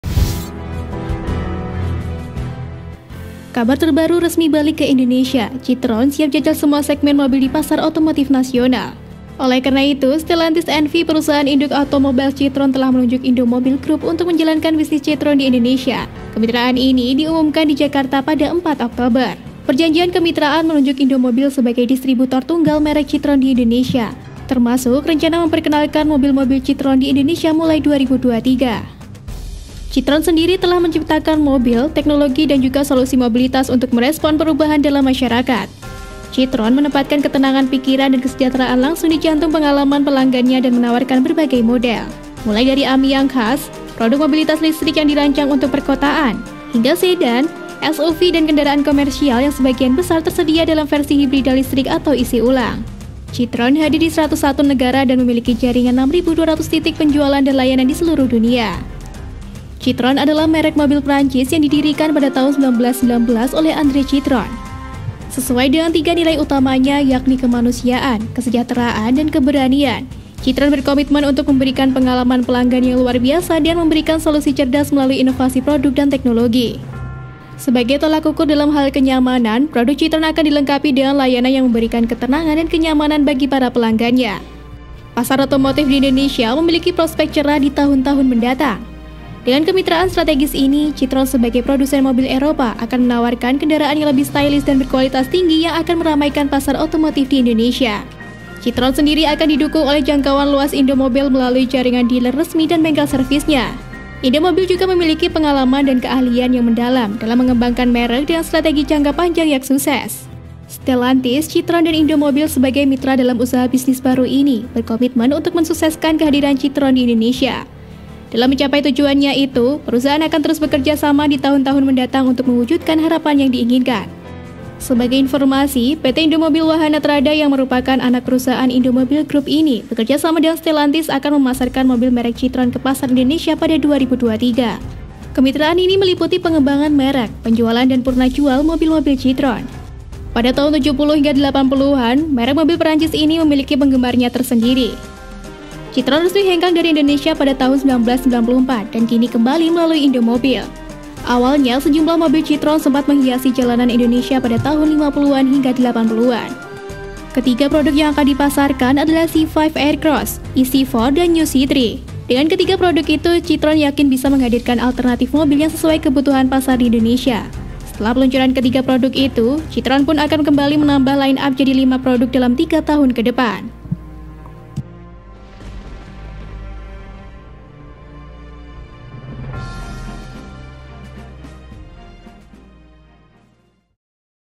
TAK TERIMA❗CITROEN KEMBALI TURUN KE JALAN RI🔥BERAMBISI KALAHKAN RENAULT & PEUGEOT DI INDONESIA❓
Mobil Citroen Kembali mengaspal di Indonesia
Apakah Dia Benar-benar Segera Menjual Produk Mobil Asal Perancis tersebut ke pasar Indonesia. Sebagai Lawan Baliknya dari, Renault & Peugeot di Indonesia!?

Kabar terbaru, resmi balik ke Indonesia, Citroen siap jajal semua segmen mobil di Pasar otomotif nasional. Oleh karena itu,  Stellantis N.V, Perusahaan induk Automobiles Citroen, telah menunjuk Indomobil Group untuk menjalankan bisnis Citroen di Indonesia. Kemitraan ini diumumkan di Jakarta 4 oktober.

Perjanjian kemitraan menunjuk Indomobil sebagai distributor tunggal merek Citroen di Indonesia, termasuk rencana memperkenalkan mobil-mobil Citroen di Indonesia mulai 2023.

Citroen telah menciptakan mobil, teknologi, dan solusi mobilitas untuk merespons perubahan dalam masyarakat. Citroen menempatkan ketenangan pikiran dan kesejahteraan langsung di Jantung pengalam pelanggannya dan menwarkan berbagai model.

Mulai dari Ami yang khas, produk mobilitas listrik yang dirancang untuk perkotaan, hingga sedan, SUV dan kendaraan komersial, yang sebagian besar tersedia dalam versi hibrida listrik atau isi ulang. Citroen hadir di 101 negara dan memiliki jaringan 6.200 titik penjualan dan layanan di seluruh dunia.

Citroen adalah merek mobil Prancis yang didirikan pada tahun 1919 oleh Andre Citroën. Sesuai dengan tiga nilai utamanya, kemanusiaan, kesejahteraan, dan keberanian, Citroen berkomitmen untuk memberikan pengalaman pelanggan yang luar biasa dengan memberikan solusi cerdas melalui inovasi produk dan teknologi.

Sebagai tolak ukur dalam hal kenyamanan, produk Citroen akan dilengkapi dengan layanan yang memberikan ketenangan dan kenyamanan bagi para pelanggannya.
Pasar otomotif di Indonesia memiliki prospek cerah di tahun-tahun mendatang.

Dengan kemitraan strategis ini, Citroen, sebagai produsen mobil Eropa akan menawarkan kendaraan yang lebih stylish dan berkualitas tinggi yang akan meramaikan pasar otomotif di Indonesia.

Citroen akan didukung oleh jangkauan luas Indomobil melalui jaringan dealer resmi dan bengkel servisnya. Indomobil juga memiliki pengalaman dan keahlian yang mendalam dalam mengembangkan merek dengan strategi jangka panjang yang sukses.

Stellantis, Citroen, dan Indomobil, sebagai mitra dalam usaha bisnis baru ini, berkomitmen untuk mensukseskan kehadiran Citroen di Indonesia. Dalam mencapai tujuan itu, perusahaan akan terus bekerja sama di tahun-tahun mendatang untuk mewujudkan harapan yang diinginkan.

Sebagai informasi, PT Indomobil Wahana Trada yang merupakan anak perusahaan Indomobil Group ini  bekerjasama dengan Stellantis akan memasarkan mobil merek Citroen ke Pasar Indonesia pada 2023.

Kemitraan ini meliputi pengembangan merek, penjualan dan purna jual mobil-mobil Citroen. Pada tahun 70-80an merek mobil Perancis ini memiliki penggemarnya tersendiri. Citroen resmi hengkang dari Indonesia pada tahun 1994 dan kini kembali melalui Indomobil.

Awalnya sejumlah mobil citroen sempat menghiasi jalanan indonesia pada tahun 50-an hingga 80-an.

Ketiga produk yang akan dipasarkan adalah C5 Aircross, e-C4, dan New C3.
Dengan ketiga produk itu, Citroen yakin bisa menghadirkan alternatif mobil yang sesuai kebutuhan pasar di Indonesia.

Setelah peluncuran ketiga produk itu, citroen pun akan kembali menambah line up jadi lima produk dalam tiga tahun ke depan.
SUMBER : Beberapa Sumber
PENULIS : Tim Naratif
Naratif adalah kanal youtube yang mengulas tentang update berita baik di indonesia. Berita hari ini dan tadi malam yang menggembirakan akan diulas disini.